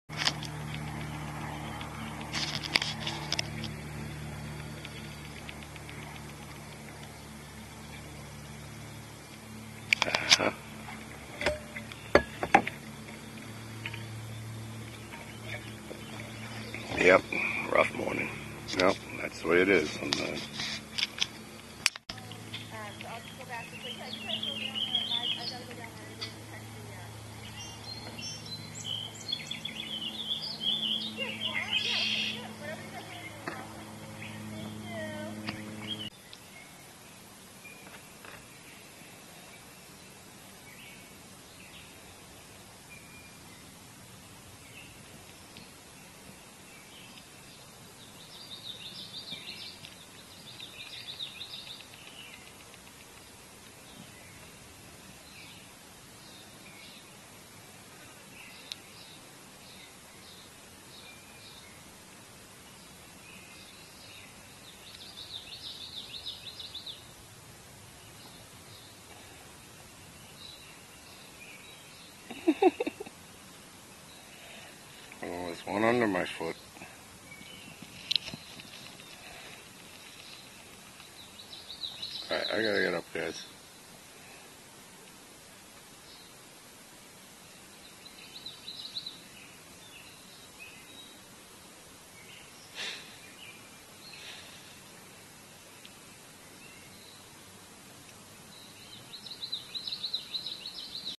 A Sunday morning float - kayaking Flints Pond in Hollis, NH
Sunday morning and I hadn't finished coffee yet.  Decided to take it with me and go look at a local pond that somehow I have not paddled in all the years I have been here.  Too small potatoes?  Never!  Every little pocket of water you can paddle is home to some wildlife.  And I was in the mood to go meet 'em.  So out to Hollis I went, where I met Beth, the president of the Flint Pond Improvement Association, and Sarah, a volunteer water monitor (and a third shy gal who didn't intro herself.. hi to ya if you're reading this!) as they slowly patrolled the pond to mark suspected milfoil spots.
I had a nice lazy cruise around the pond.  Sipped coffee, witnessed a turtle stampede and got a short pedi courtesy of some friendly fish at the launch ramp.  Nice place to unwind a little!

Want to get out and paddle the northeast?  Join us in the New England Kayak Club!  We're on Meetup as well as Facebook.